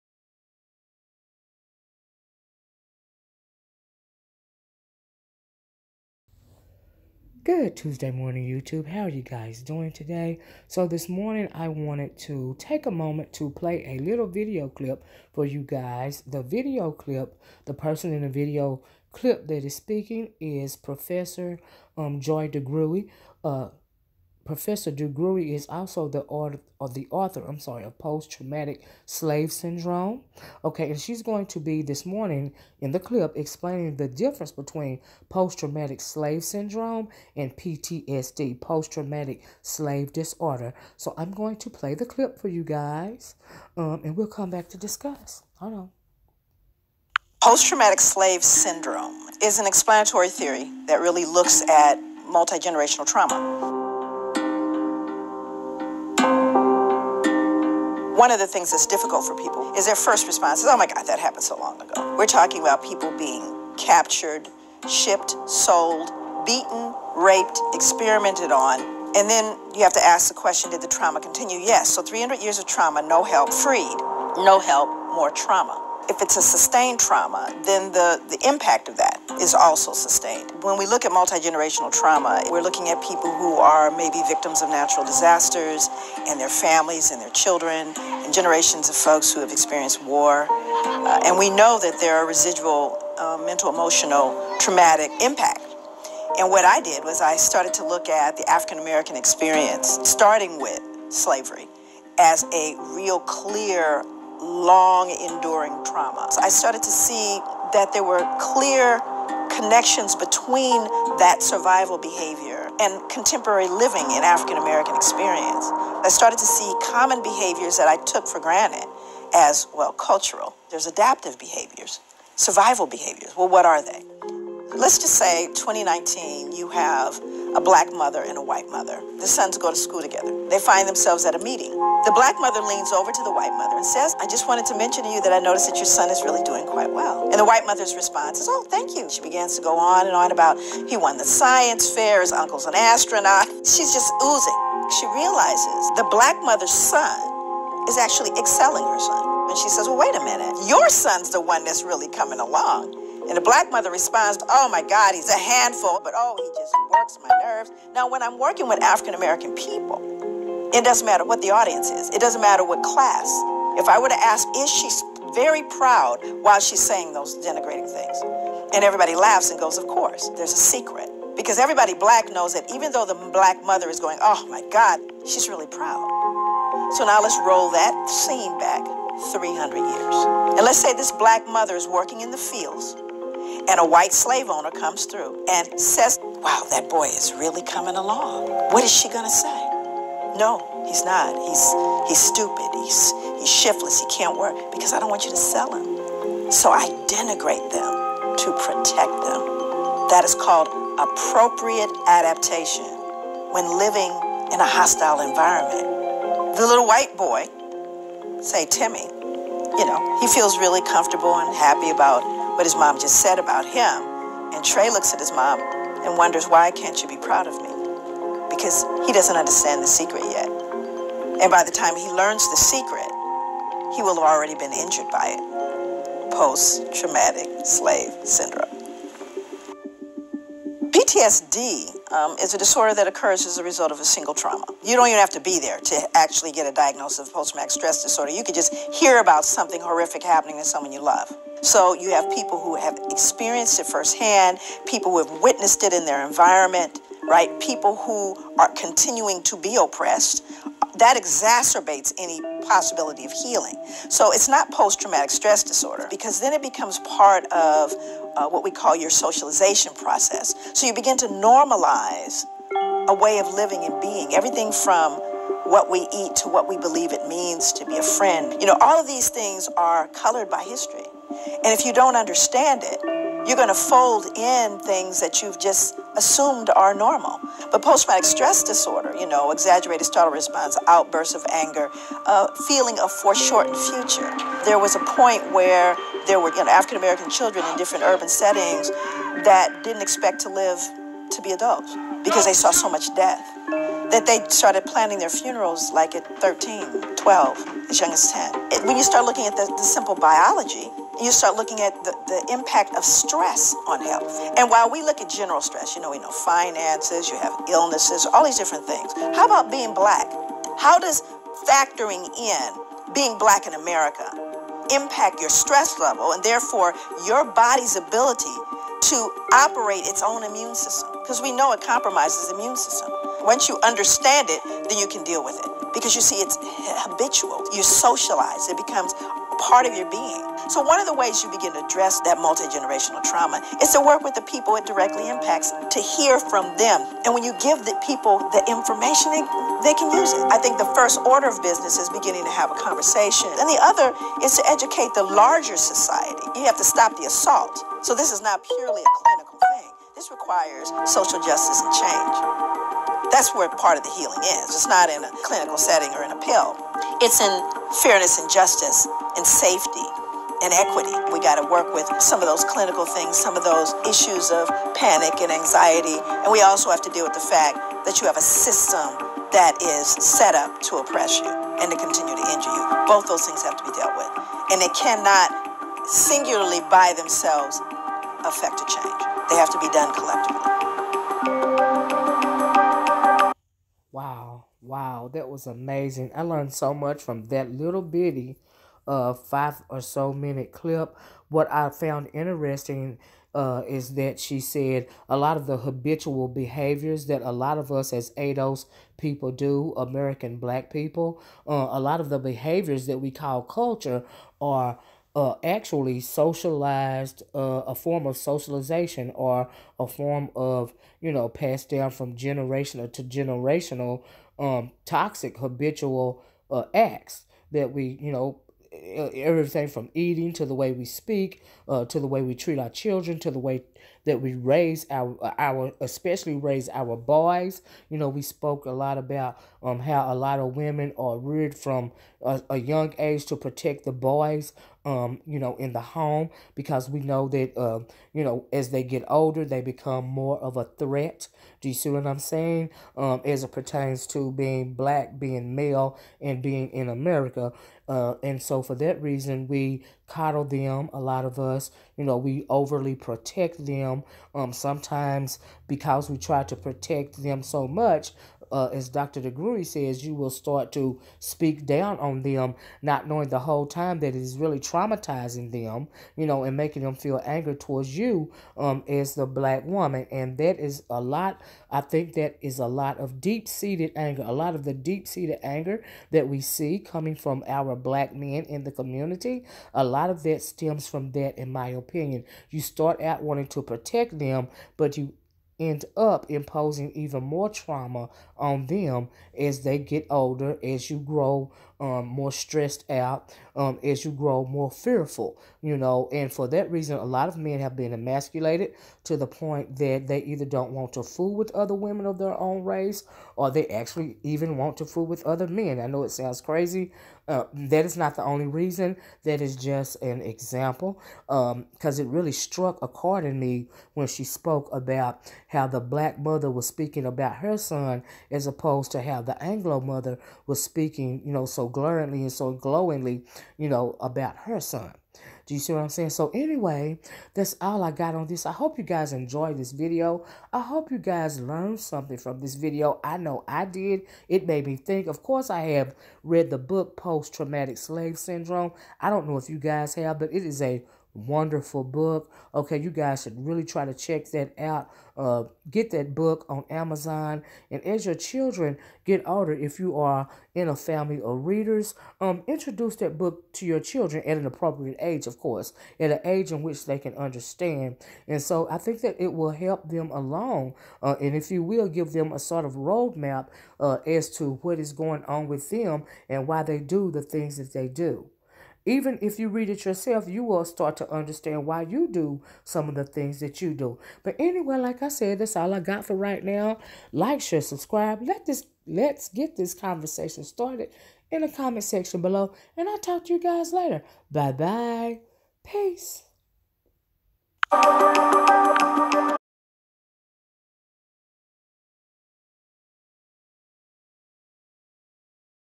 Dr. Joy DeGruy discussing the difference between Post Traumatic Slave Syndrome and PTSD
This video is intended for Entertainment and Educational purposes only.